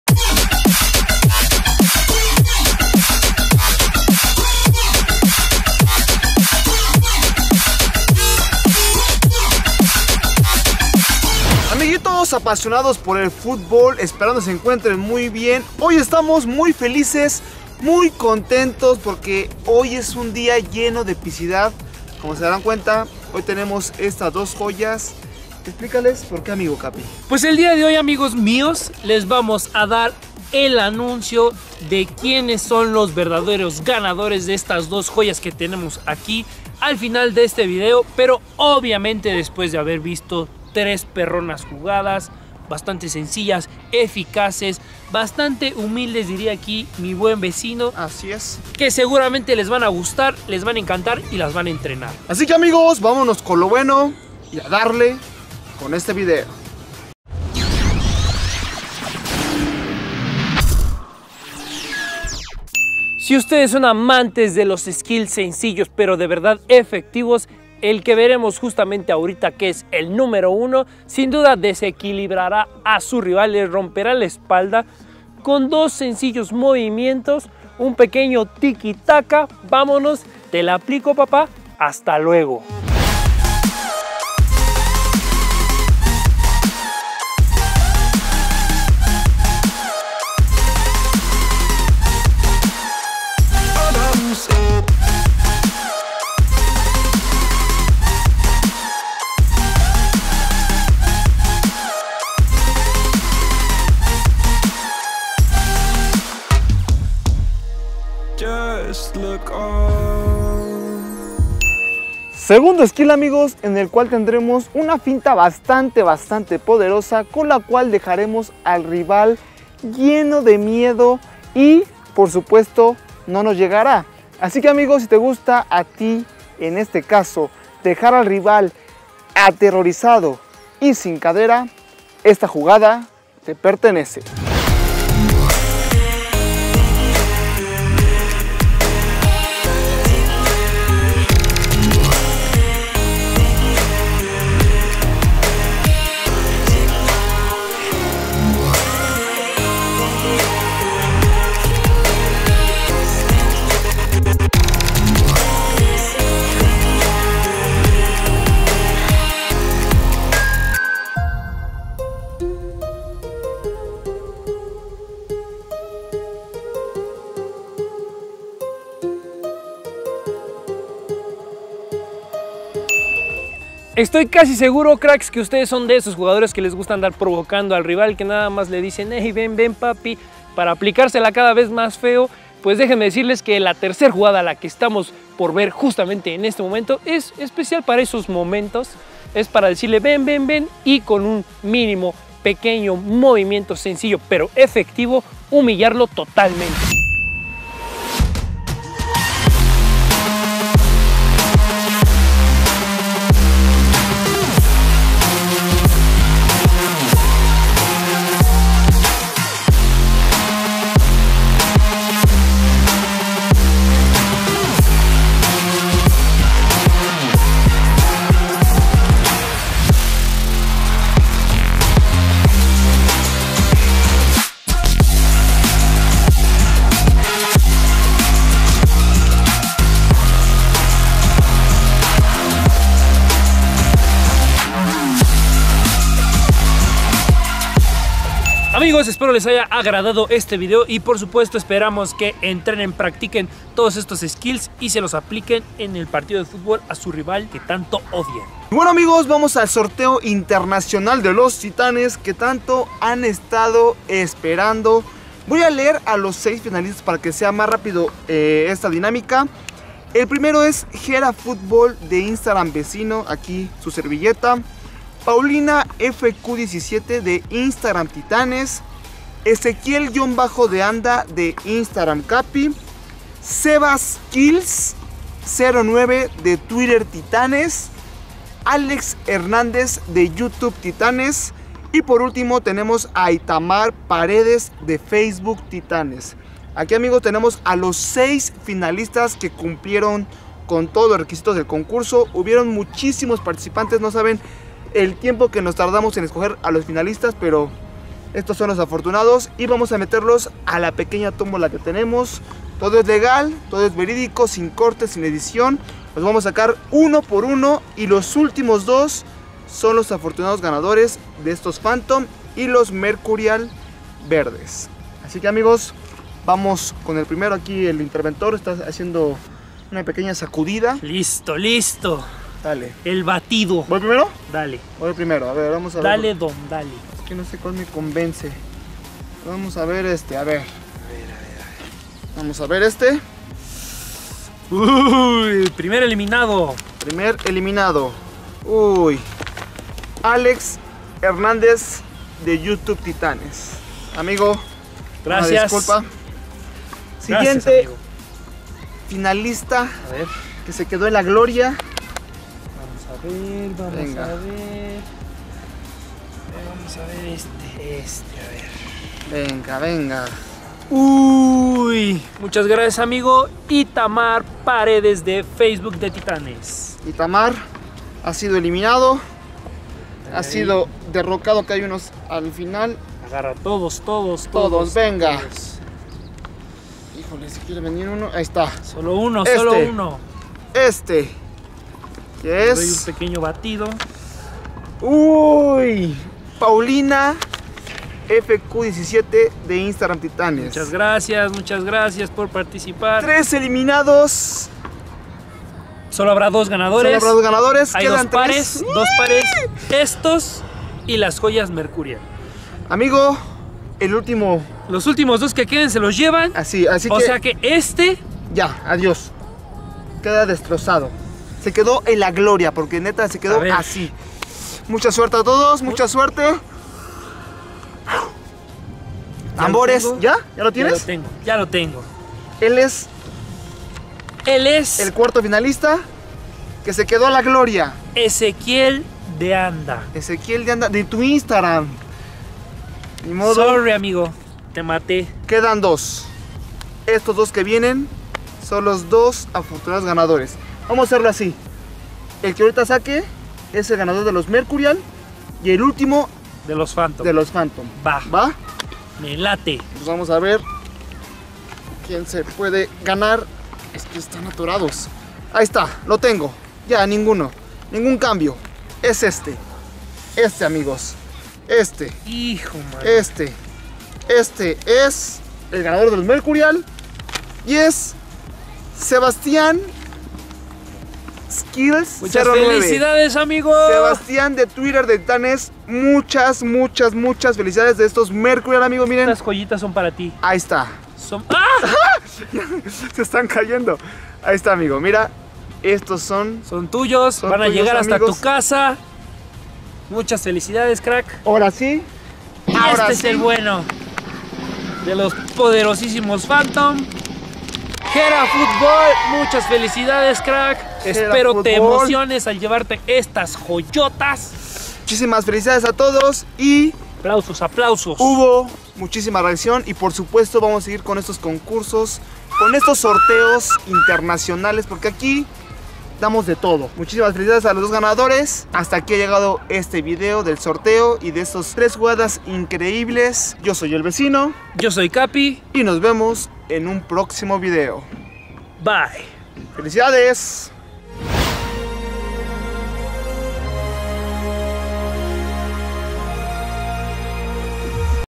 ESTOS son los 3 REGATES MAS EFECTIVOS de BURLAR a tu RIVAL en el FÚTBOL de manera SORPRENDENTE ⚽️
VIDEO ANTERIOR ►